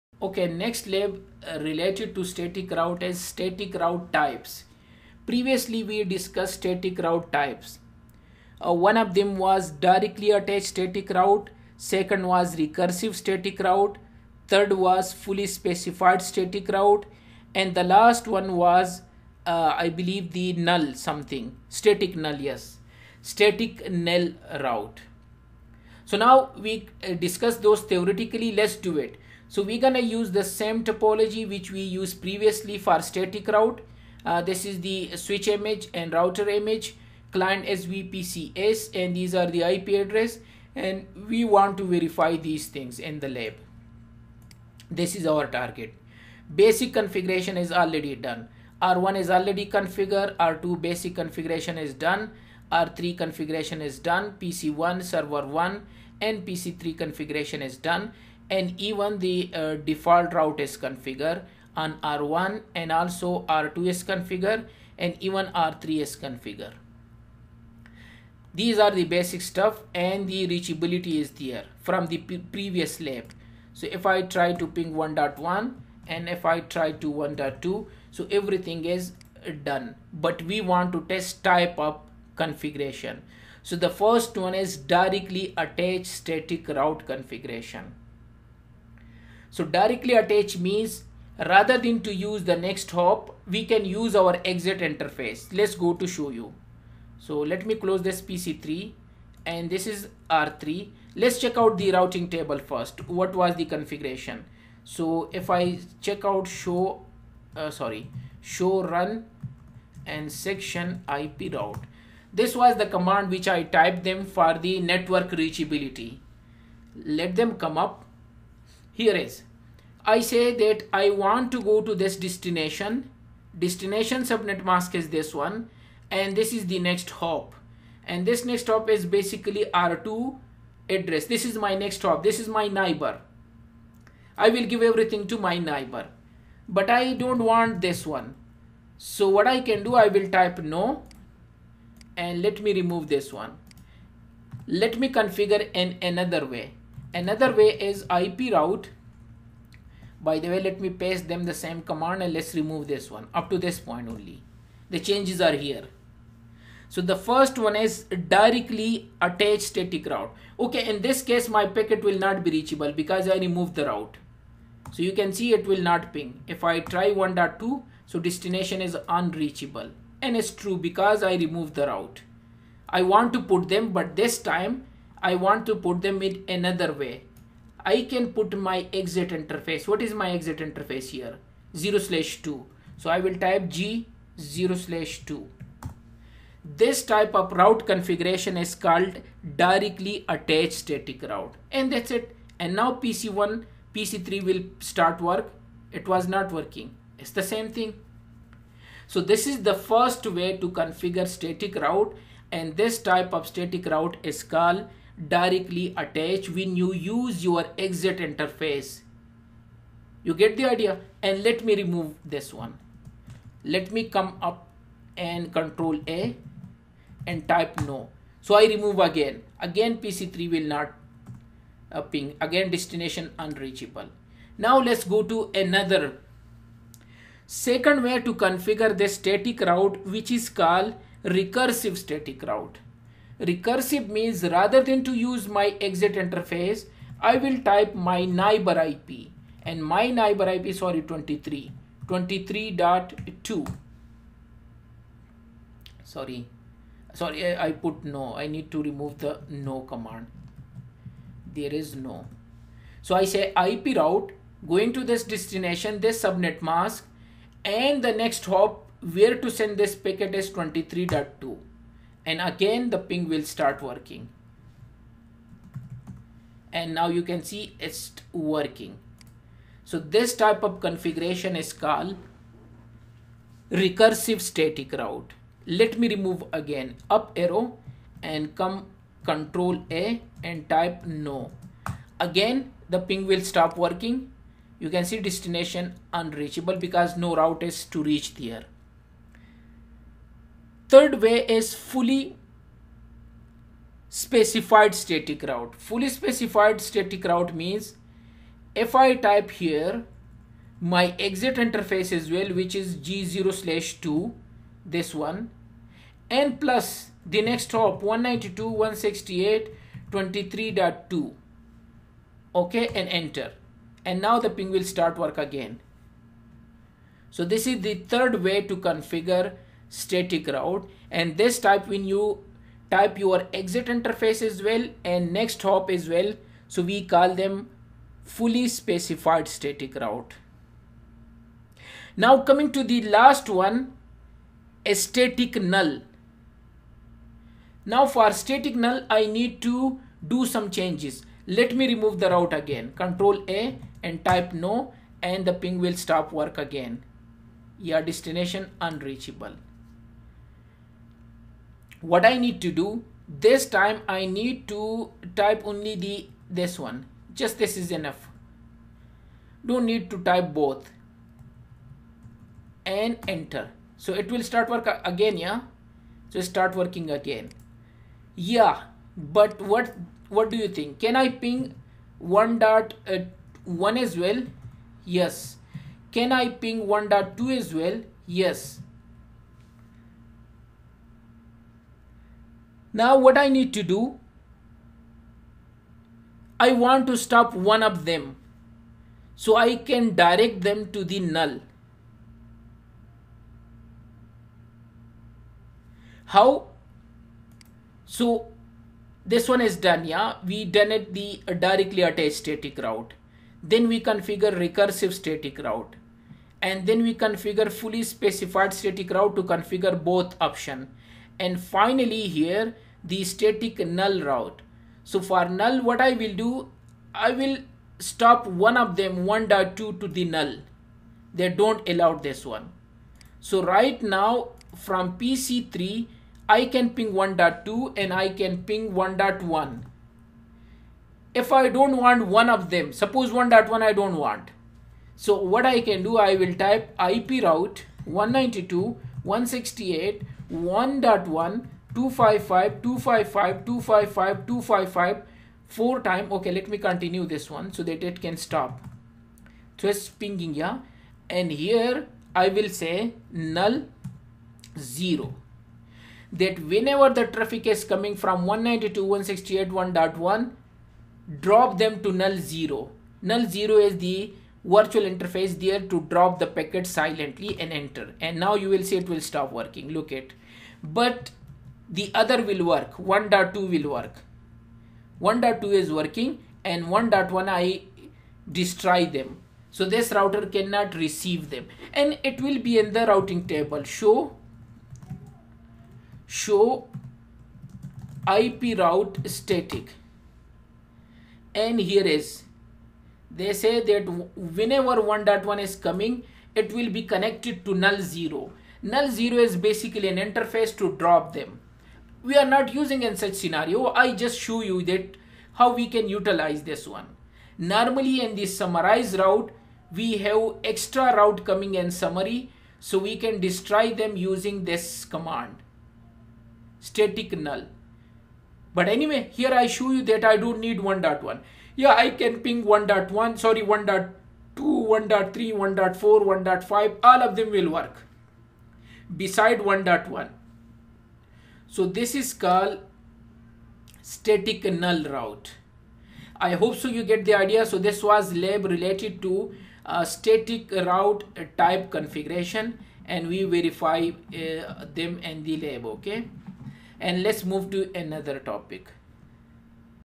CCNP-59:Configure & Verify Static Routing Routes Types
Static Route Types:
Static routes can be classified as one of these Directly Attached Static Routes, Recursive Static Routes, Fully Specified Static Routes and Static Null Routes.